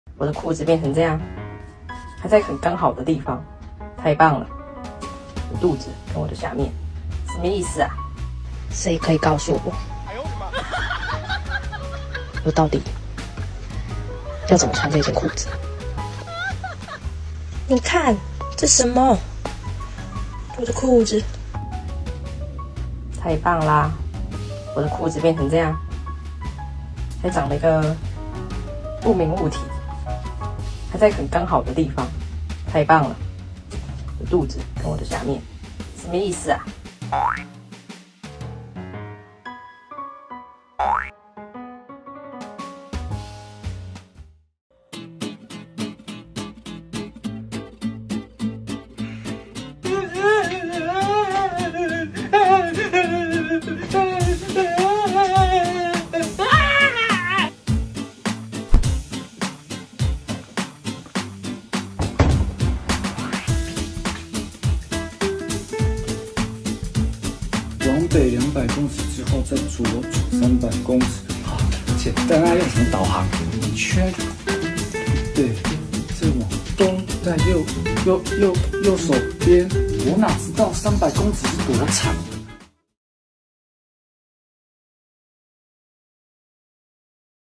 老爸幫女兒曬褲子 衣架撐歪...凸起形狀超害羞｜搞笑｜尷尬
☑影片授權：TikTok davina1101